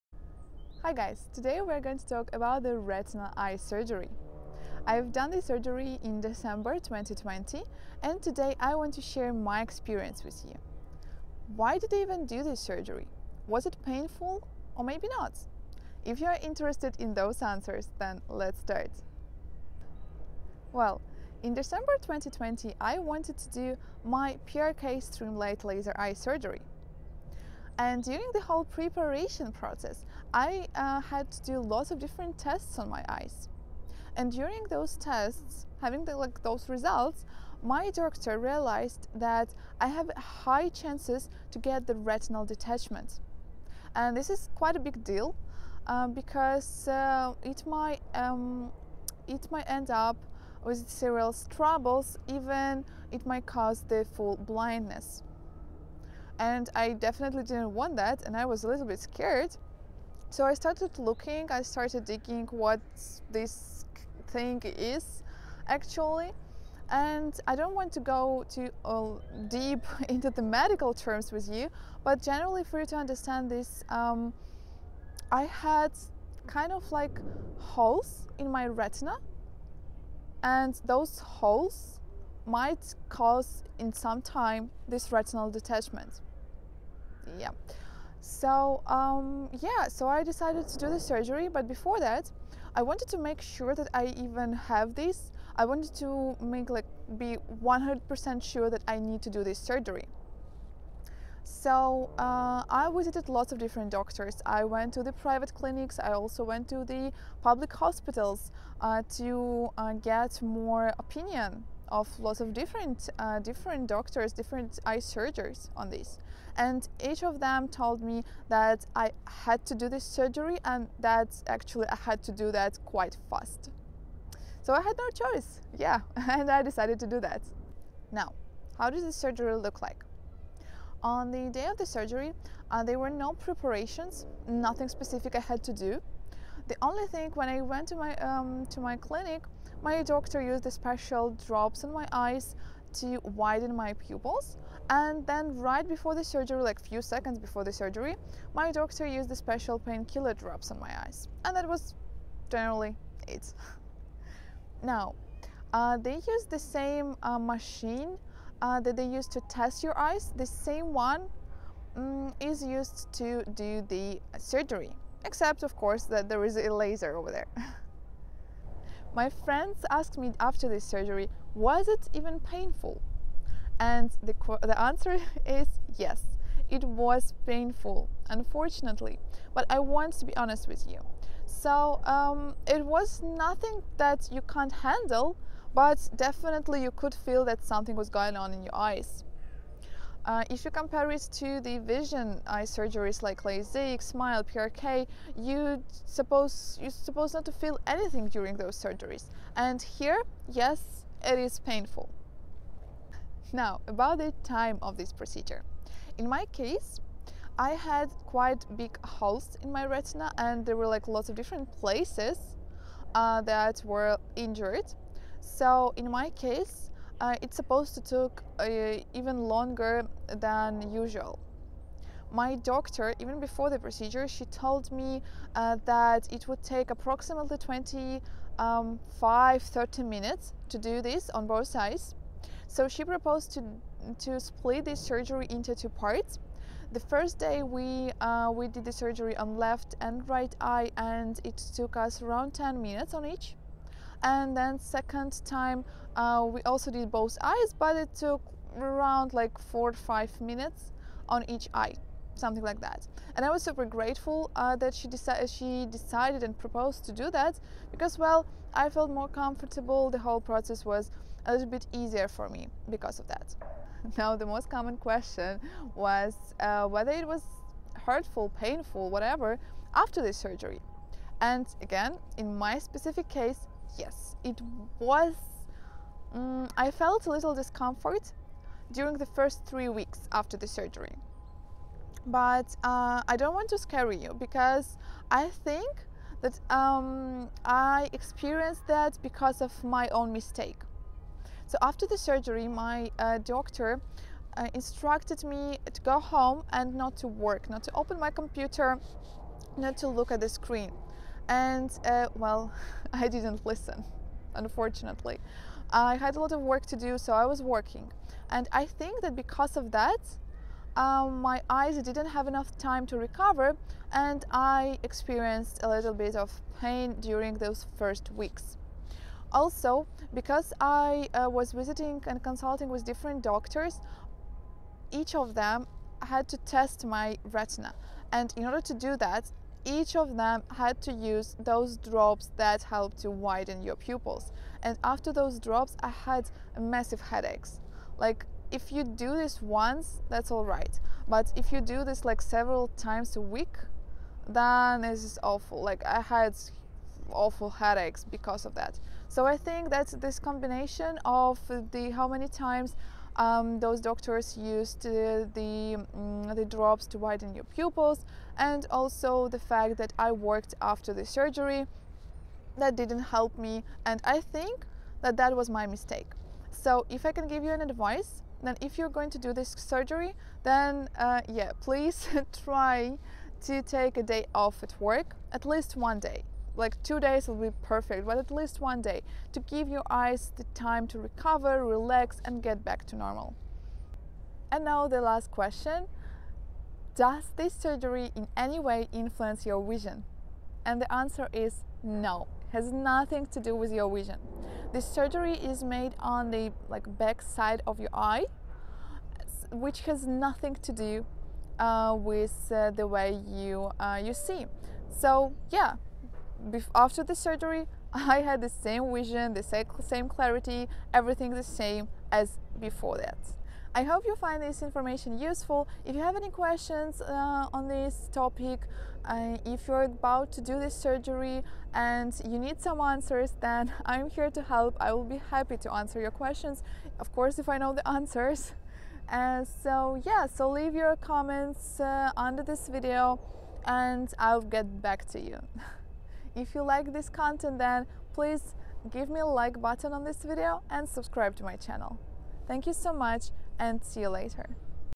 Retinal laser eye surgery experience - photocoagulation. How to prevent retinal detachment
Hello, dears!
I've done my Retinal laser photocoagulation eye surgery in December 2020.
In this video, I want to share my experience with you by answering the most common questions on retinal photocoagulation surgery!

Video agenda:
00:00 - introduction
00:19 - Why did I have to do this treatment?
02:07 - How does the surgery look like?
02:48 - Laser photocoagulation - is it painful?
03:23 - How long does the procedure take?
04:23 - Retinal photocoagulation laser eye surgery recovery
06:24 - What effect does laser photocoagulation have on the clarity of your vision? Doest it have any?

I have also done the PRK Streamlight Laser eye surgery and recorded a few videos on that topic as well :)

I would be happy to help if you have any questions regarding the procedure. I am not a doctor, but I can share my experience with you.